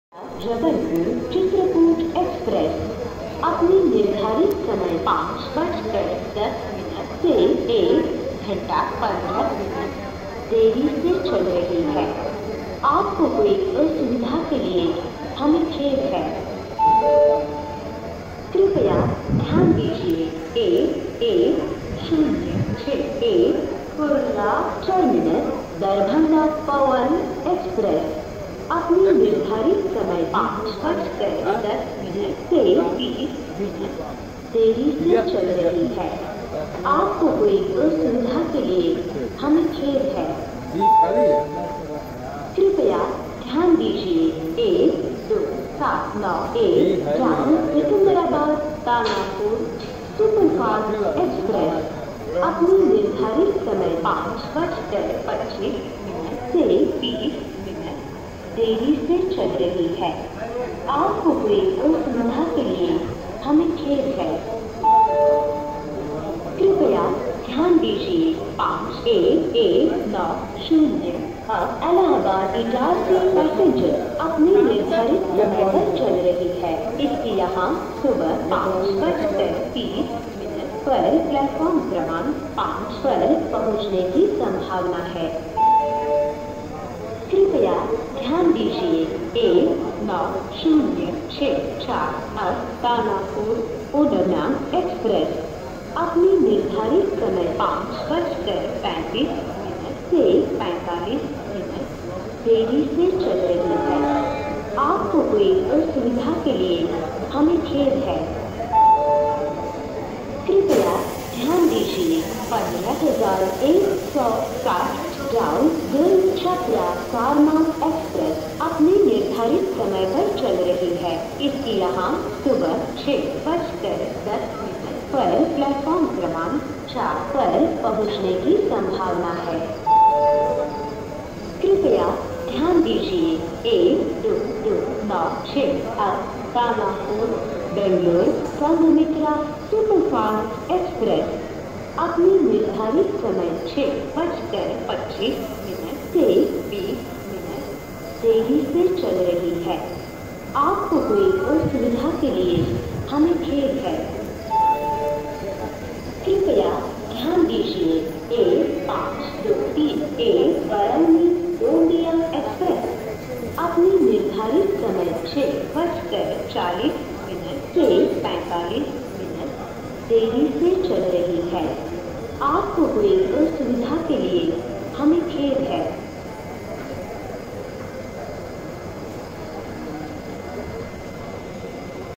KATNI STATION LONGEST TRAIN ANNOUNCEMENTS AT EARLY MORNING -ANAS KHAN IRFCA/MSTS
Announcement at Katni Junction Railway Station of Katni in the early morning features,